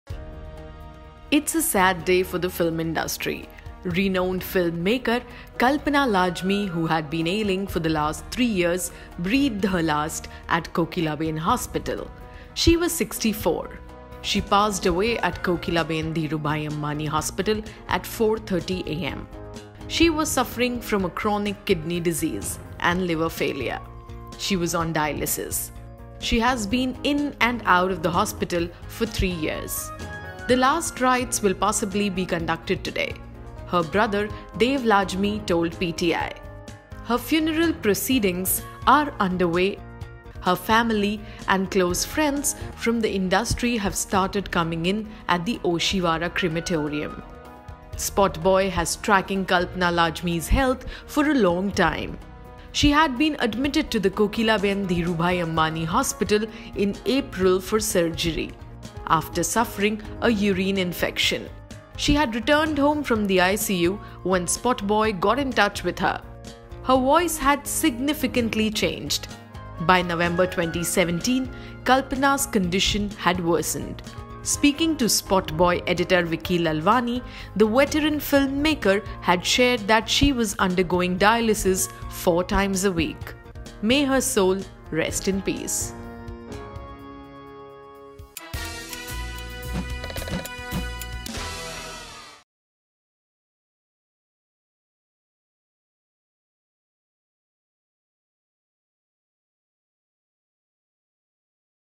Kalpana Lajmi Funeral: Heartbroken Shabana Azmi And Soni Razdan Attend The Filmmaker’s Last Rites
Here are the first pics from the funeral ceremony of noted filmmaker Kalpana Lajmi, who left for her heavenly abode earlier this morning, at 4:30 am. It's a sad day for the film industry